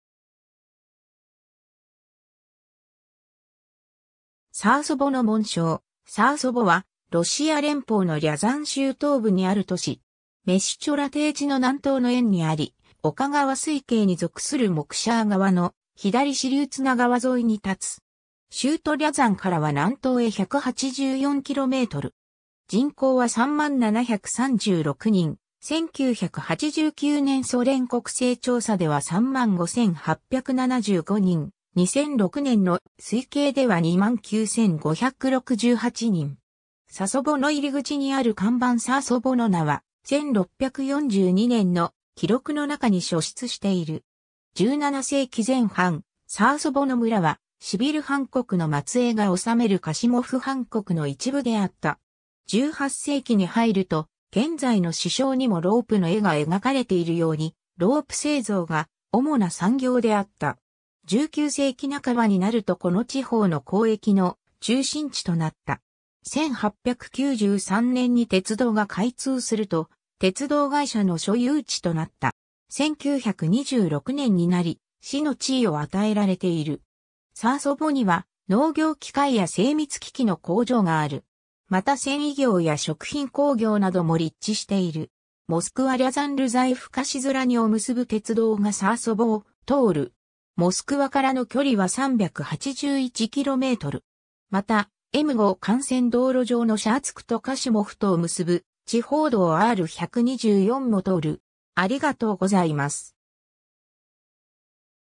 サーソヴォ
サーソヴォの紋章 サーソヴォ（ロシア語: Са́сово; Sasovo）はロシア連邦のリャザン州東部にある都市。
メシュチョラ低地の南東の縁にあり、オカ川水系に属するモクシャー川の左支流ツナ川（Цна）沿いに建つ。
州都リャザンからは南東へ184キロメートル。
人口は30,736人（2002年国勢調査）、1989年ソ連国勢調査では35,875人、2006年の推計では29,568人。
 サソヴォの入口にある看板 サーソヴォの名は、1642年の記録の中に初出している。
17世紀前半、サーソヴォの村はシビル・ハン国の末裔が治めるカシモフ・ハン国の一部であった。
 18世紀に入ると、現在の市章にもロープの絵が描かれているように、ロープ製造が主な産業であった。
19世紀半ばになるとこの地方の交易の中心地となった。
1893年に鉄道が開通すると、鉄道会社の所有地となった。
 1926年になり市の地位を与えられている。
 サーソヴォには農業機械や精密機器の工場がある。
また繊維業や食品工業なども立地している。
 モスクワ＝リャザン＝ルザエフカ＝シズラニを結ぶ鉄道がサーソヴォを通る。
モスクワからの距離は381km。
また、M5幹線道路上のシャーツクとカシモフとを結ぶ地方道R124も通る。